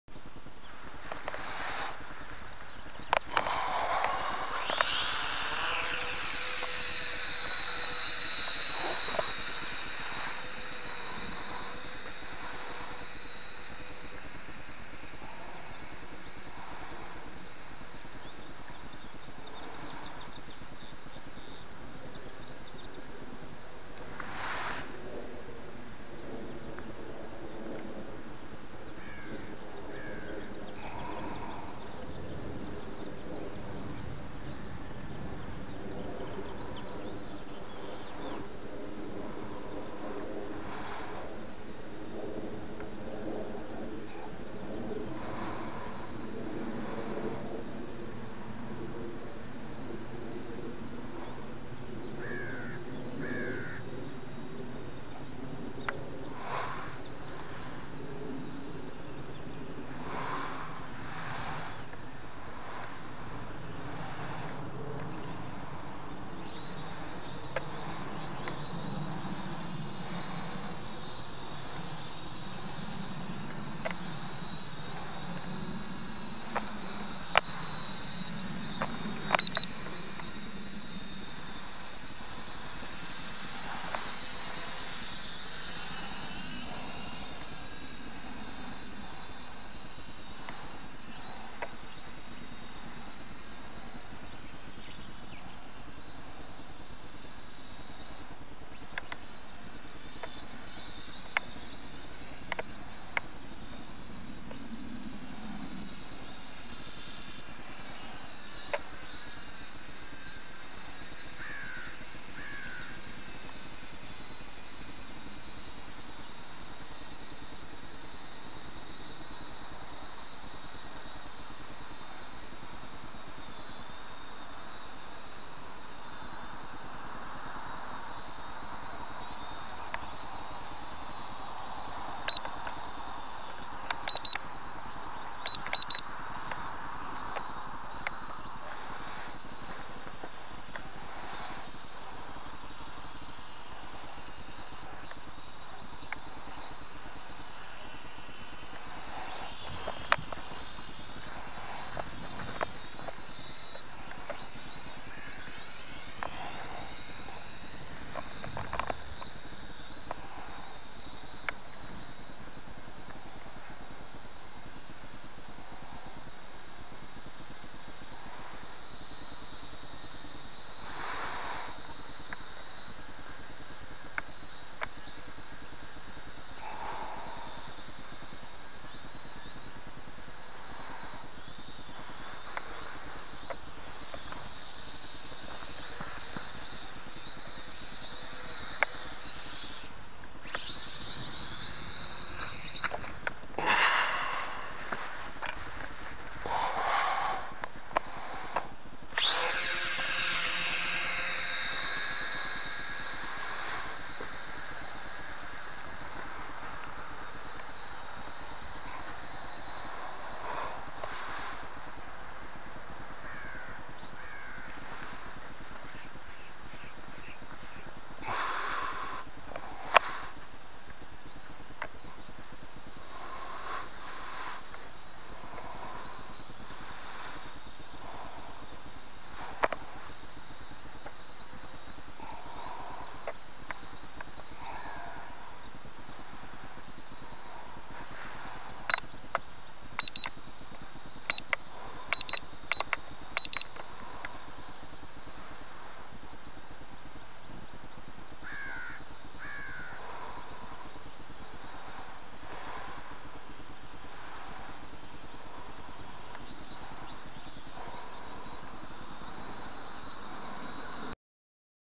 JJRC H5P Drone Antenna Modification Works
Dad helped me modify the antenna on my JJRC H5P. This was filmed on a breezy day with the landing gear and propeller props on.
Creepy Crawl with Sobaire on FACEBOOK